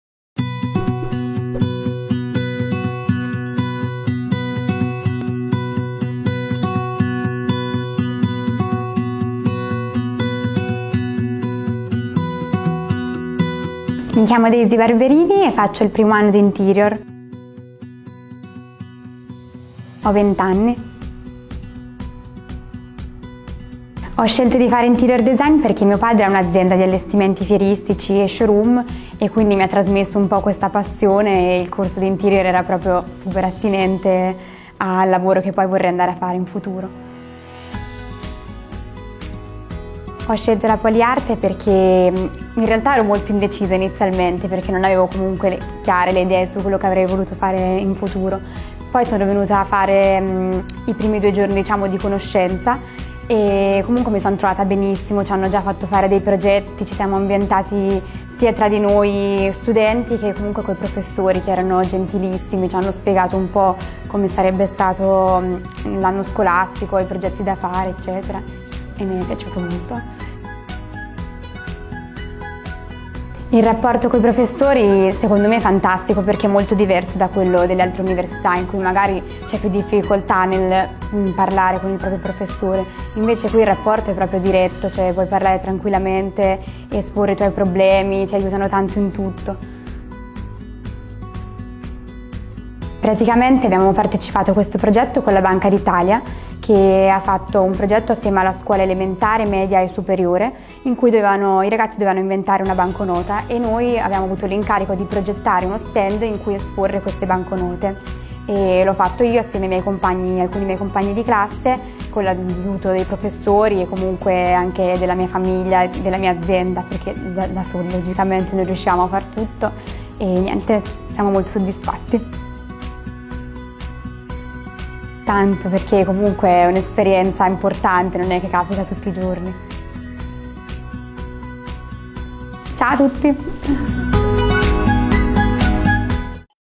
Poliarte. Gli studenti di oggi i professionisti di domani
Intervista a Daisy Barberini, studentessa del corso di laurea triennale in Interior design